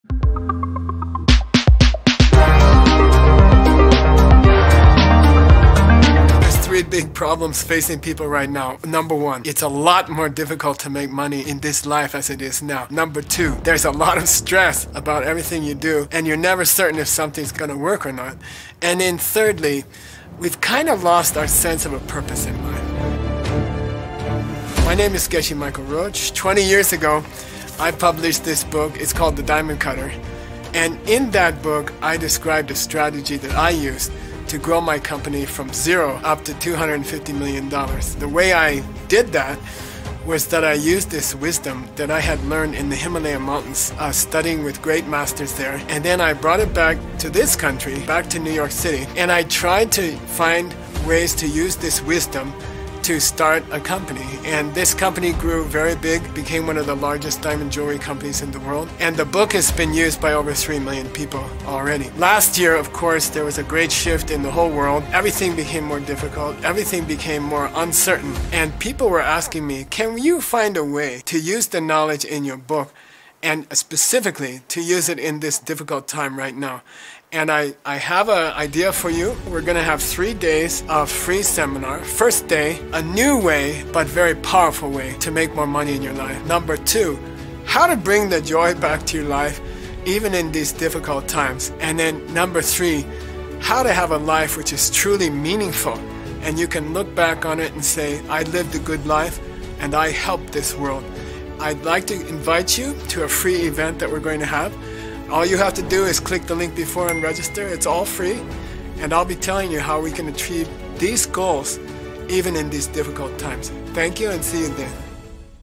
LIFE-CHANGING | FREE SEMINAR | Geshe Michael Roach | How to thrive in times of change
100% free and a life changing opportunity for you!

– 5 key strategies to increase your wealth, bring back the joy and live a deeply meaningful life even through difficult times.
– When business meets the “Diamond Cutter Wisdom”
– What kind of intelligence collision will occur between the two?
– Geshe Michael Roach First Global Wealth Public Talk
– Using the 2500 years ancient wisdom to teach you to solve the business dilemma.
– Superb management wisdom defeats uncertainty.
– Increase in wealth and strength, return to inner joy.
– 3 days public talk, live online worldwide